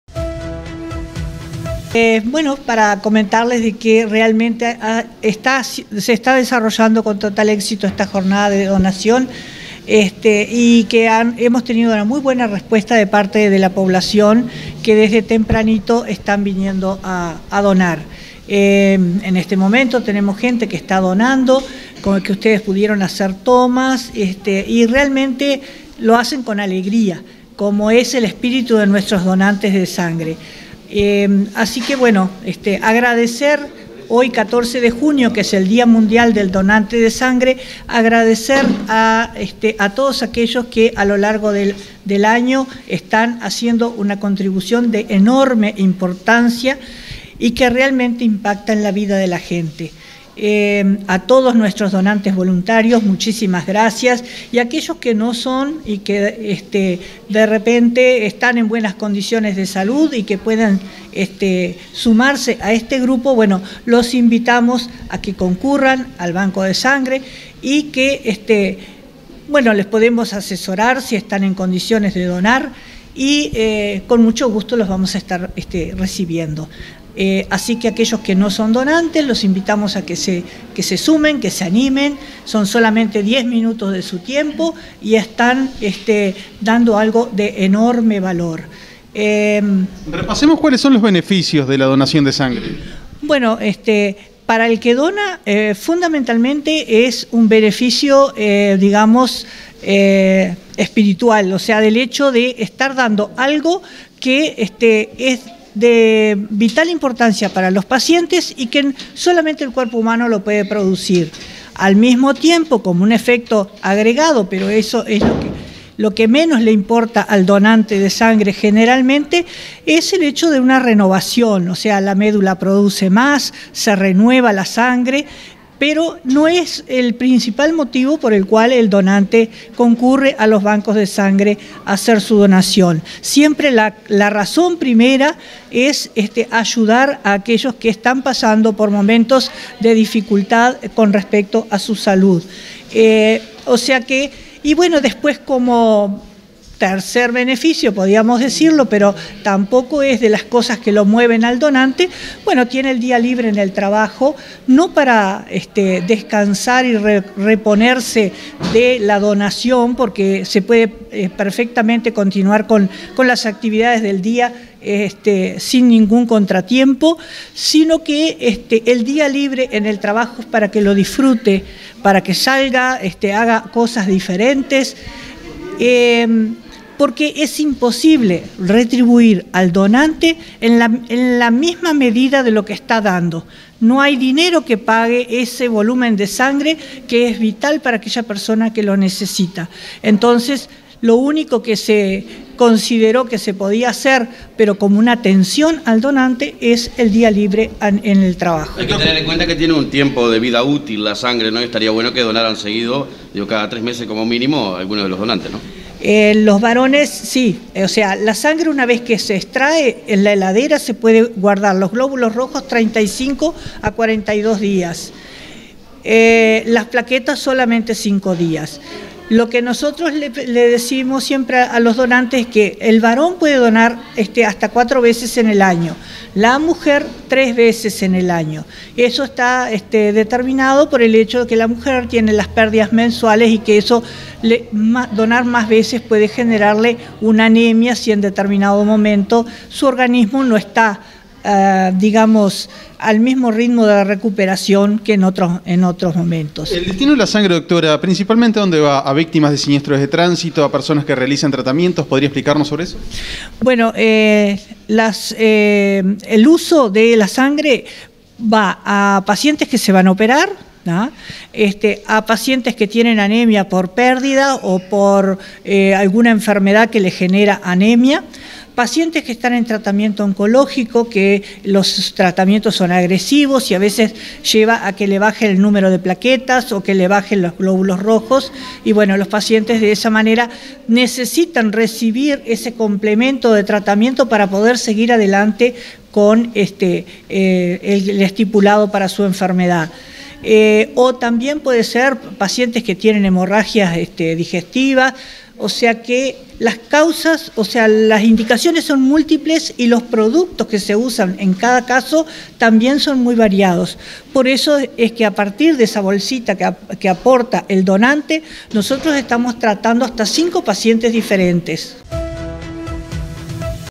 Hoy es el Día Mundial del Donante Voluntario de Sangre.
Visitamos con Mundo Visión el policlínico de COMTA